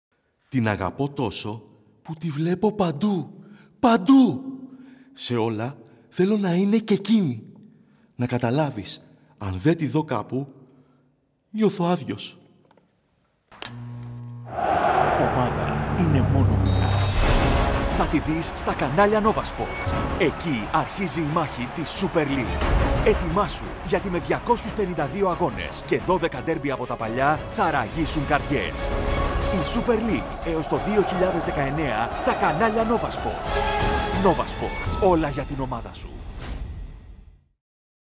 Novasports - Ομάδα είναι μόνο μία.
Άκου έναν άντρα να μιλάει για την ομάδα της καρδιάς του και θα καταλάβεις γιατί όλοι, μαζί με τα κανάλια Novasports και τη Lowe Athens, ετοιμάζονται για μια νέα σεζόν στη Super League γεμάτη συγκινήσεις, μάχες και derby. Για τη χρονιά του πραγματικού οπαδού!